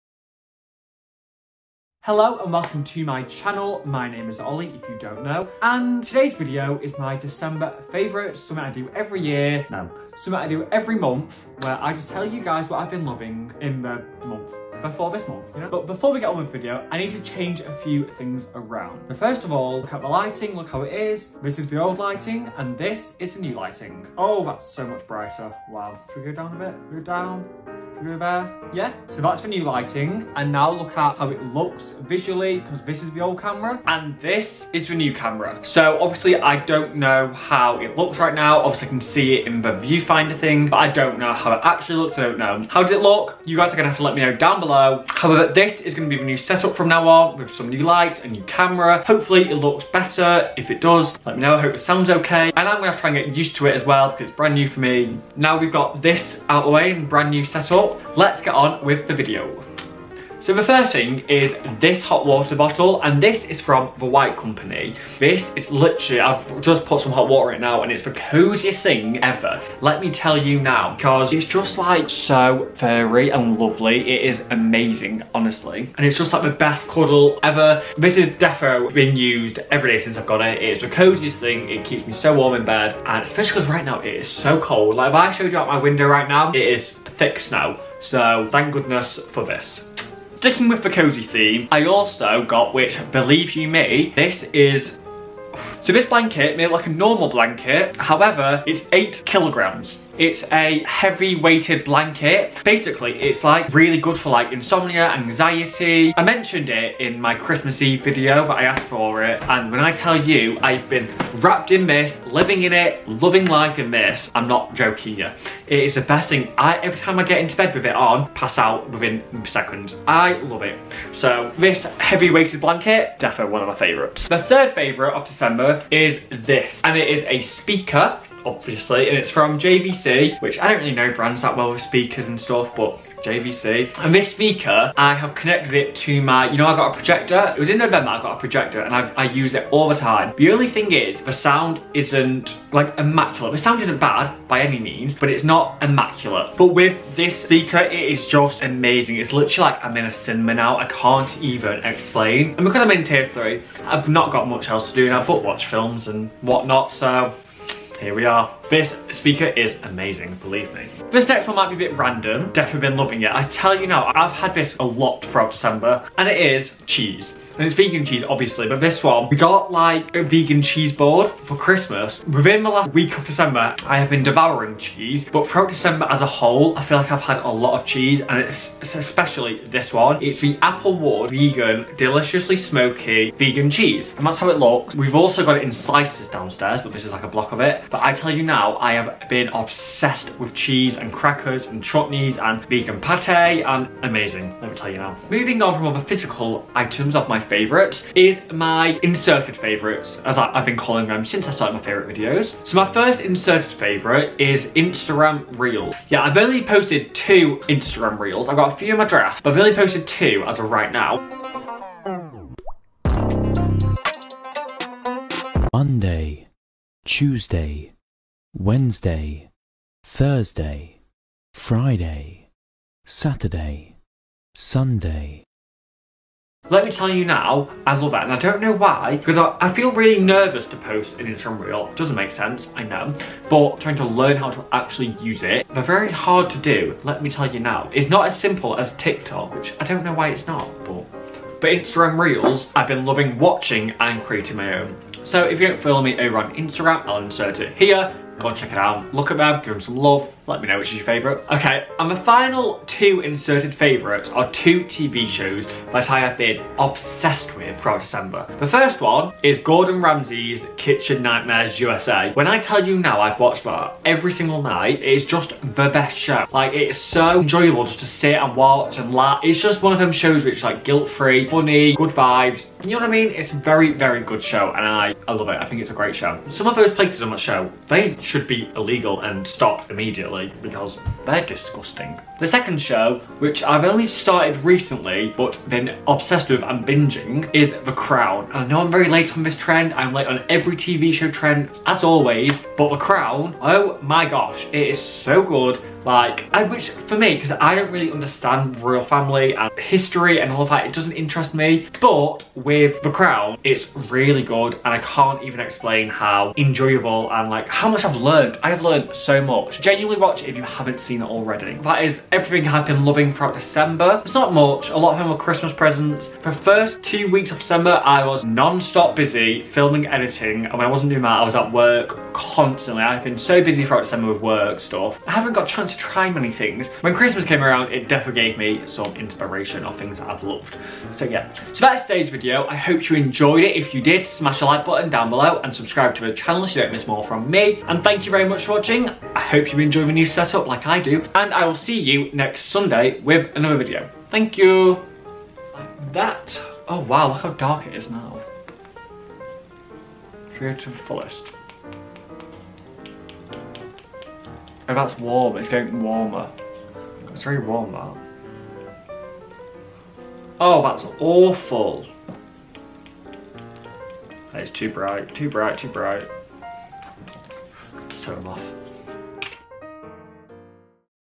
DECEMBER FAVOURITES - 2020
So its the final favourites of the year!!

Brentfords Weighted Blanket:

Gordon Ramsey Kitchen Nightmares:
Channel 4

The Crown:
Netflix